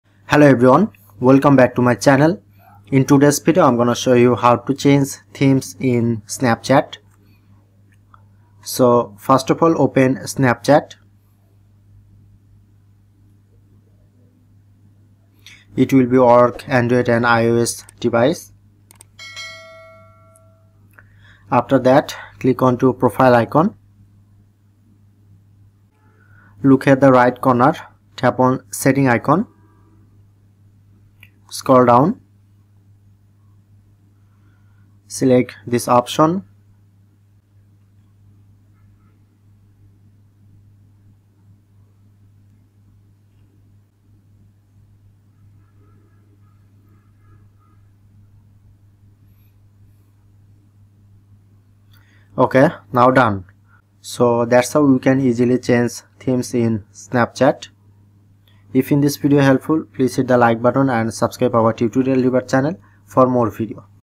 How to Change Theme in Snapchat 2026 | get Dark mode | Enable Dark Mode in Snapchat (Android & iOS)
how to Change Theme in Snapchat, how to get dark mode on Snapchat? We provide you a step by step plan for iOS and Android to tell you how you can do it

Video Parts:
0:00 Info & Intro
0:10 Open Snapchat
0:28 Go To Settings icon
0:59 Ending & Outro